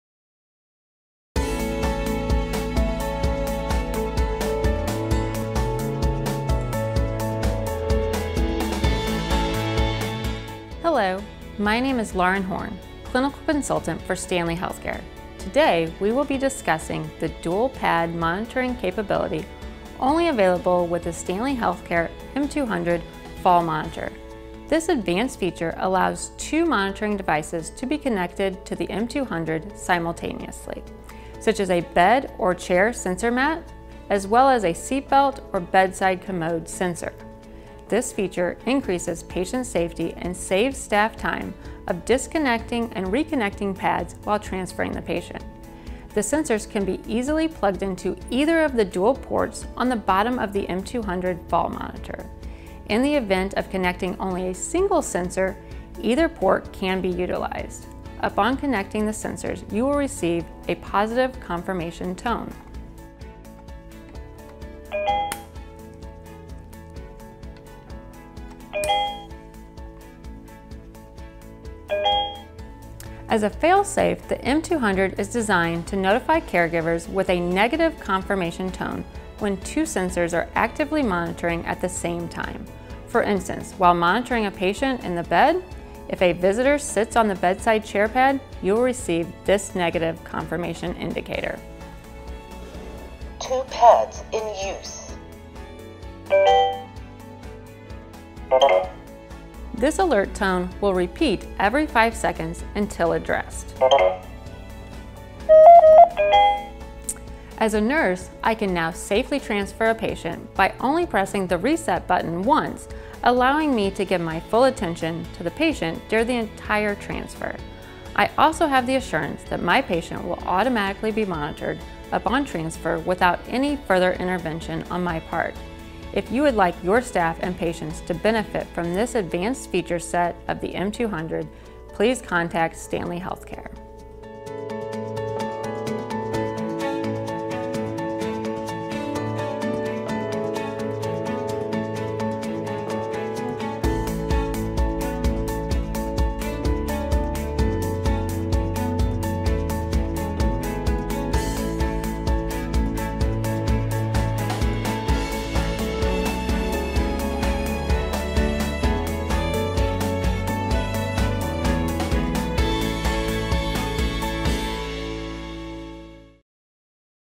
M200 Fall Management | Dual Pad Capability | STANLEY Healthcare
Discuss with Lauren Horn RN, MSN the benefits of utilizing the M200 Fall Monitor's dual pad capability for advanced safety. Simplify staff workflows and enhance patient protection with this unique functionality offered by STANLEY Healthcare.